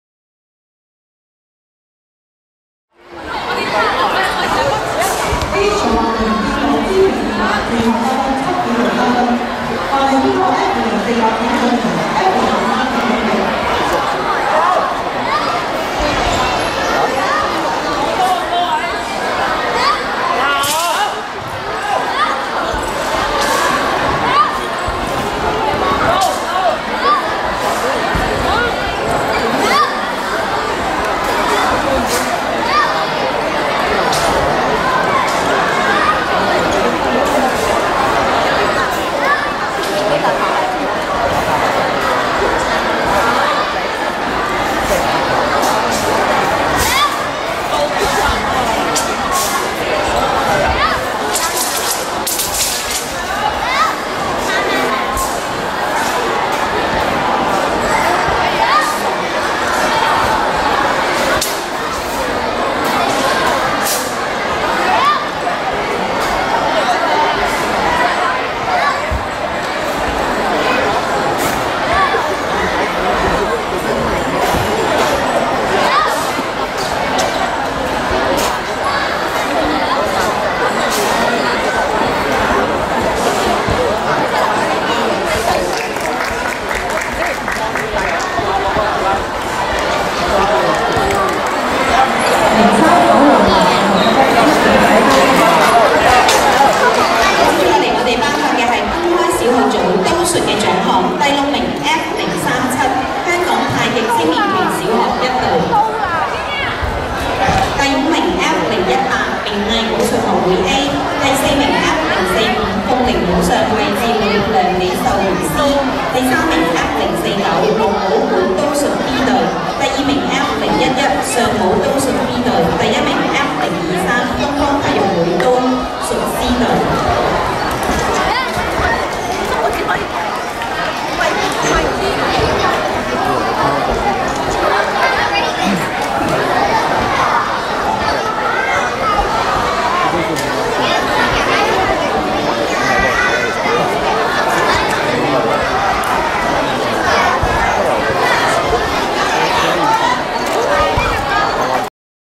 龍武館~公開小學組初級刀術B隊季軍 (2017全港公開新秀武術錦標賽) HD
Video presented by Wonderfoto @ NAM

2017全港公開新秀武術錦標賽

龍武館 Dragon Wushu Academy

Head Coach : 劉汝沁師父 Mary Lau
Videographer : Nam Ho
Video Editors : Nam Ho
Video Support : Mandy Lo